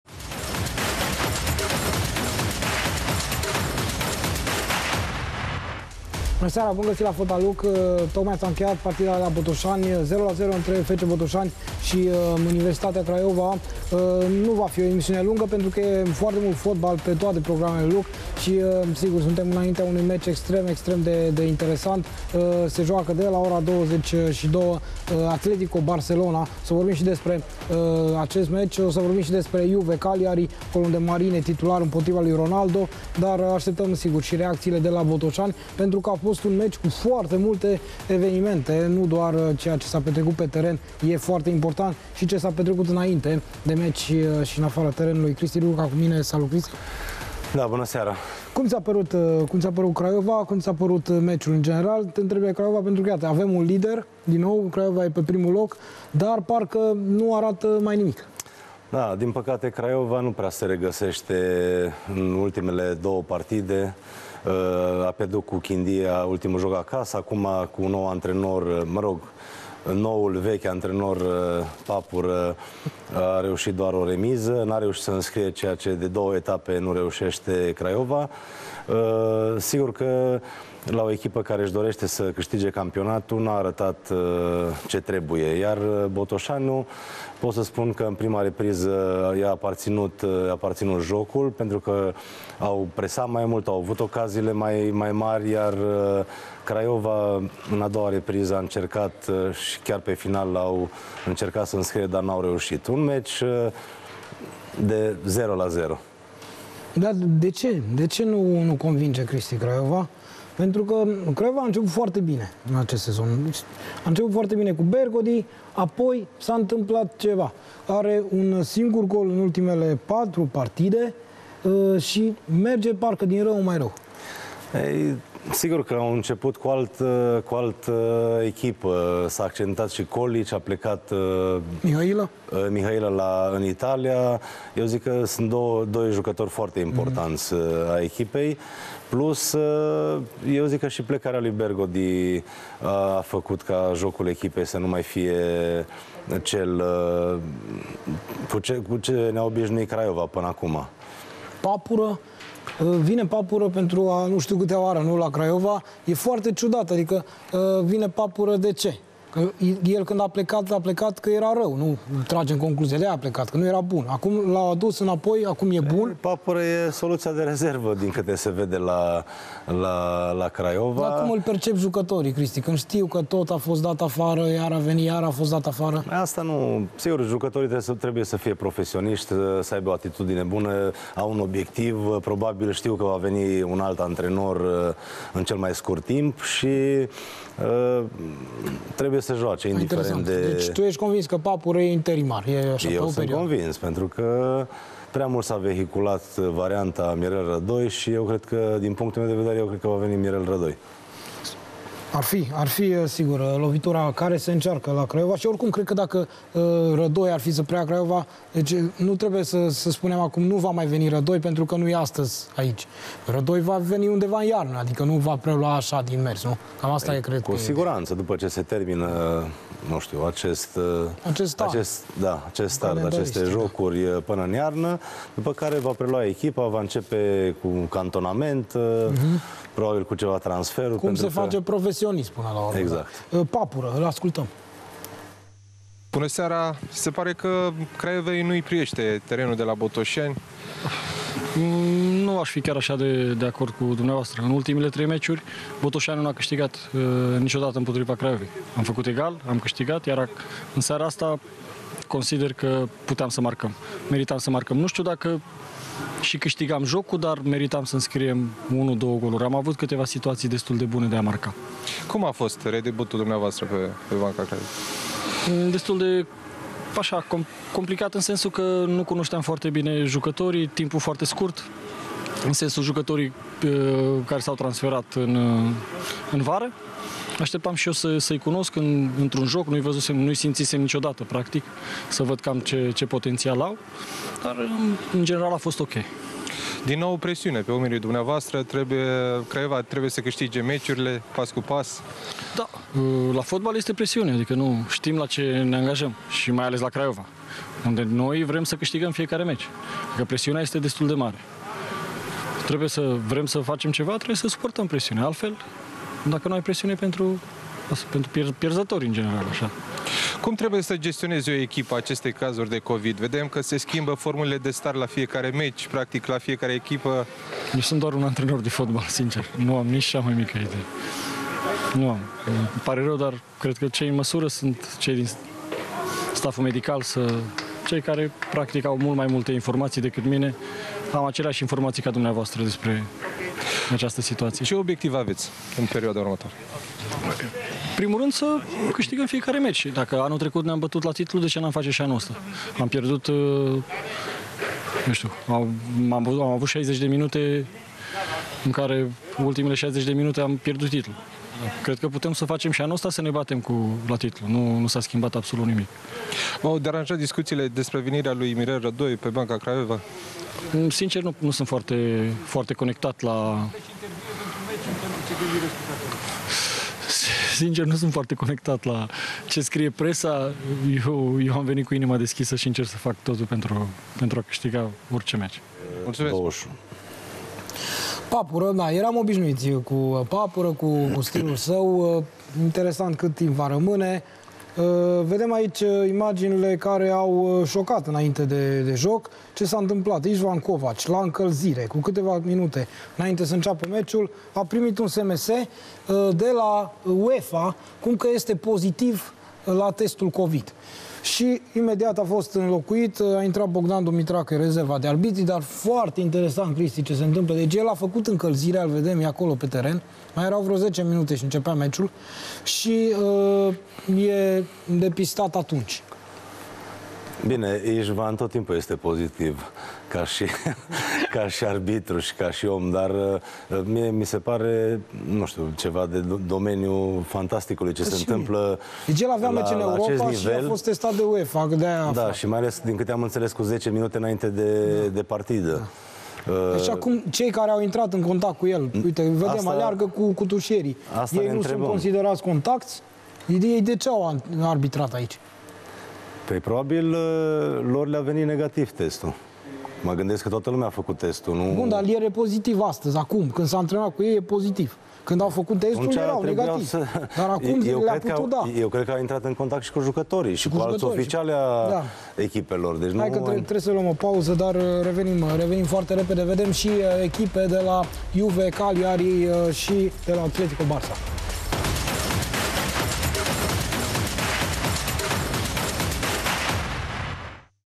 Fotbal Look. Şoc la Botoşani: Arbitrul Istvan Kovacs a aflat în vestiar că are coronavirus
"Centralul" a efectuat încălzirea înaintea partidei dintre Botoşani şi Craiova, iar când a revenit la vestiare a aflat că testul la coronavirus i-a ieşit pozitiv.